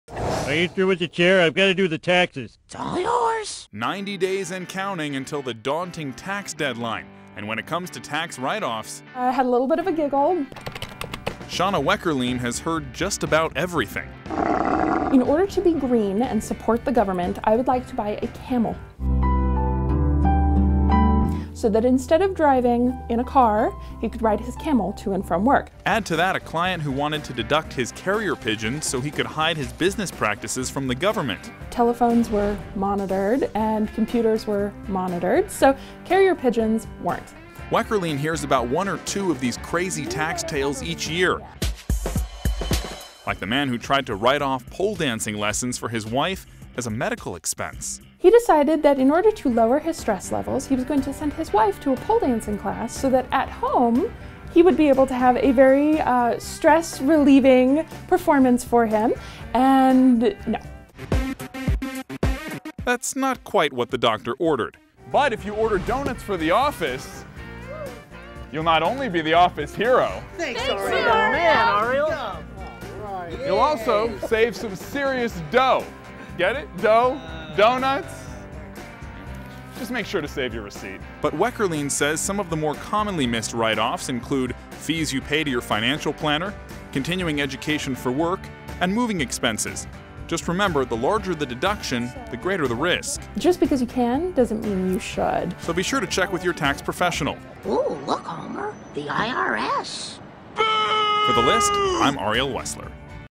Crazy Tax Write Offs
It's said there are two certainties in life:  Death and taxes. Turns out, you can only get an automatic extension on one of those. So, if you have to pay taxes, why not take as many legal deductions as you can?